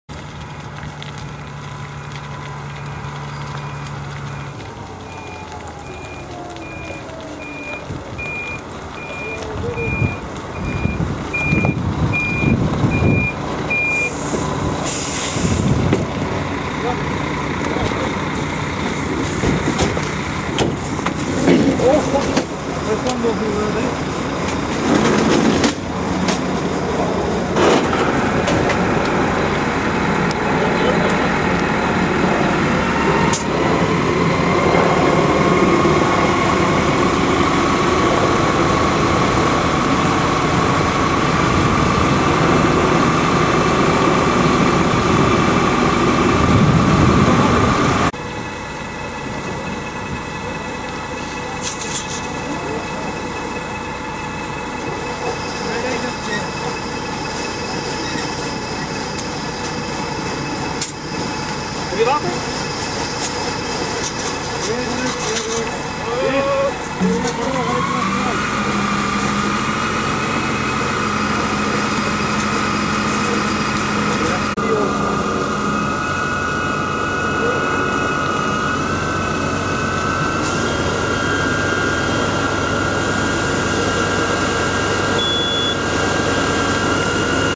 Fikse brand op Boulevard Rozenburg
Op de Boulevard in Rozenburg is op Zondag 1 september een brand ontstaan.
De brand is aangestoken door kinderen die na aankomst van de Politie de nabij gelegen wijk zijn in gerend.
De Politie heeft de kinderen nog niet kunnen pakken.
Film: Michael van der Wagt